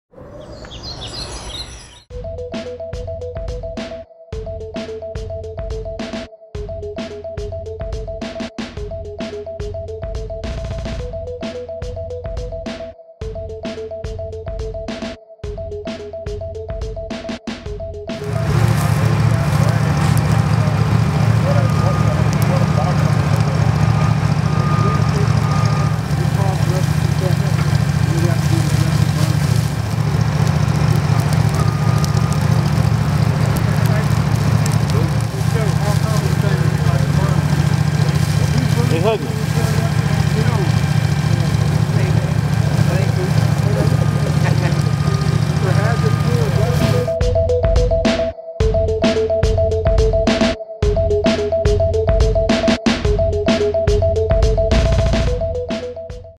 SFE Video: Osceola National Forest Prescribed Fire
Video of a prescribed fire in longleaf pine flatwoods in the Osceola National Forest near Olustee, Florida. Recorded April 2015. Recorded as part of a Southern Fire Exchange workshop and field tour hosted by the Okefenokee Osceola Longleaf Implementation Team and the Osceola National Forest.